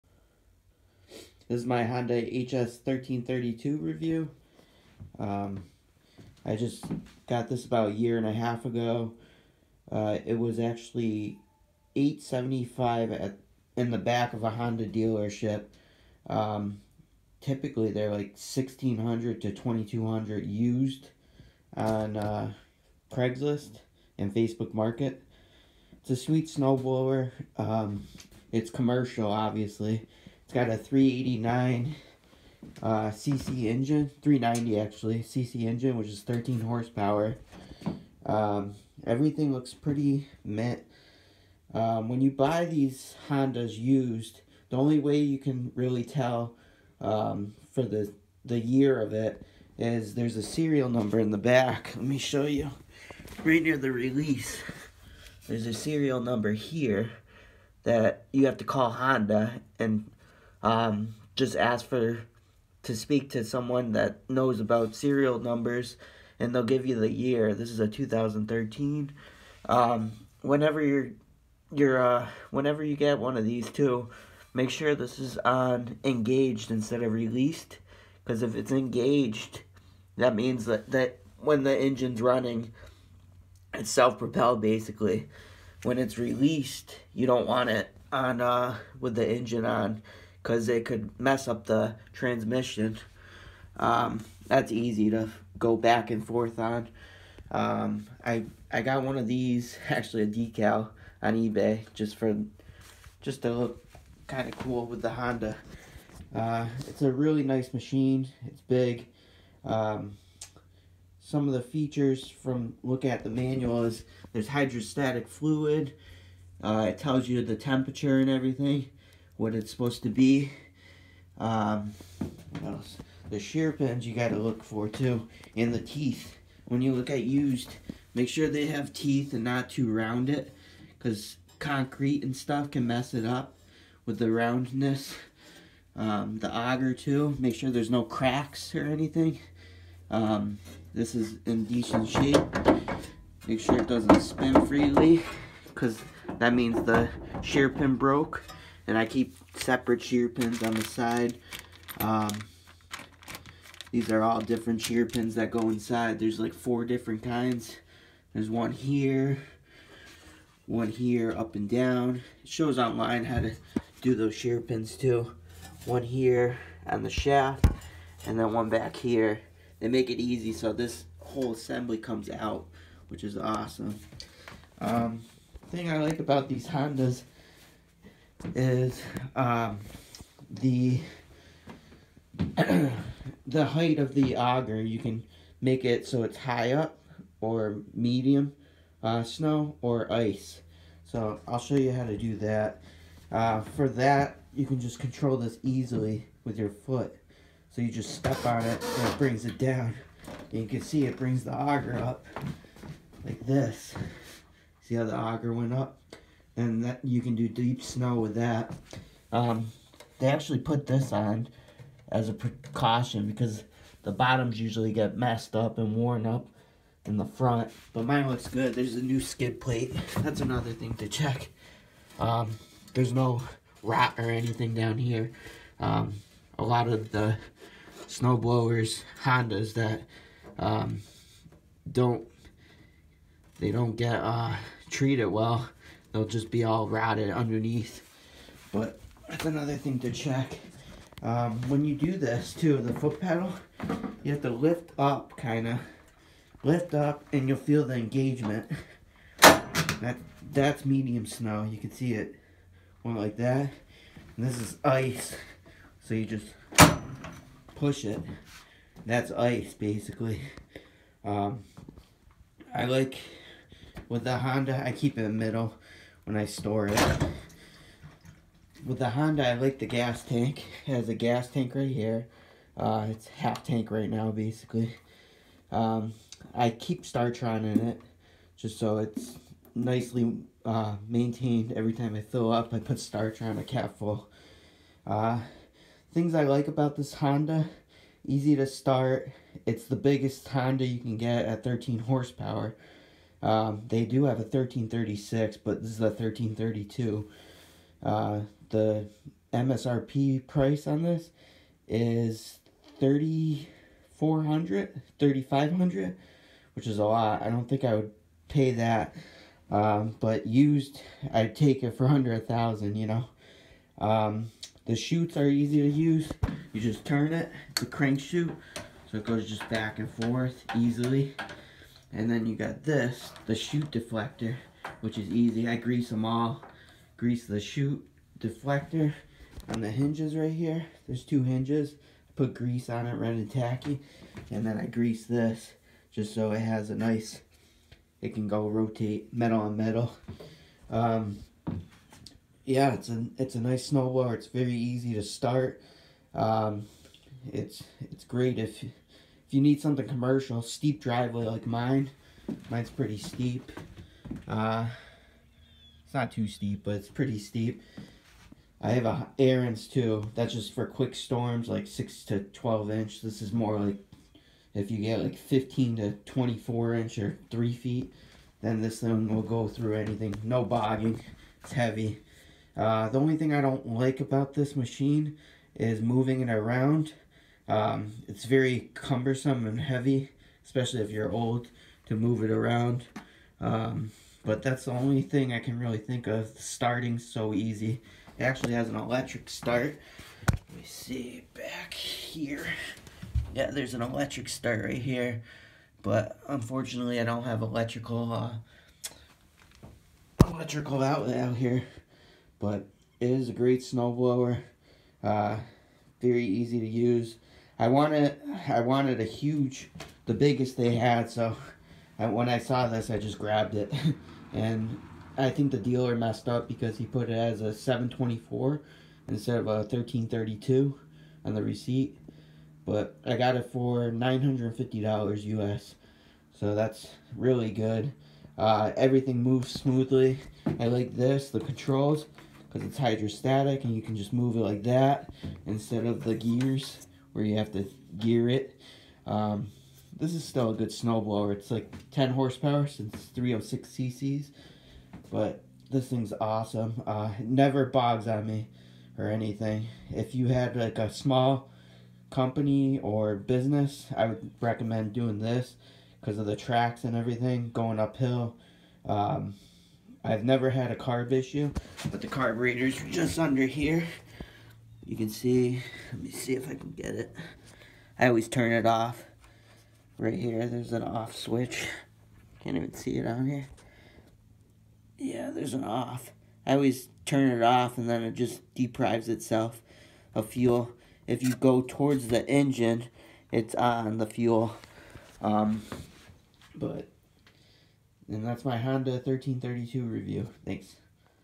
Used Honda Snowblower Buyers Guide and Review
Video on my Honda HS1332 Review. What to look for and what to have as spare.

Checklist when buying used Honda:
-auger teeth and skid plate
-hydrostatic fluid level
-starts (should be first or second pull)
-inspection of tracks And deflection
-shear pins and inside auger housing
-chute and chute control
-forward and reverse speed of the snowblower
-oil and oil level
-gas does it look or smell old? Any water in it